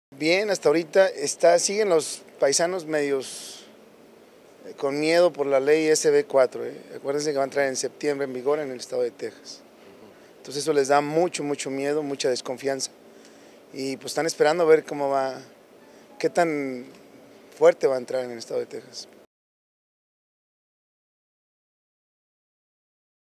Dip Roberto Alejandro Segovia Hernandez
Autoridades deben prever programas de apoyo ante posible retorno de paisanos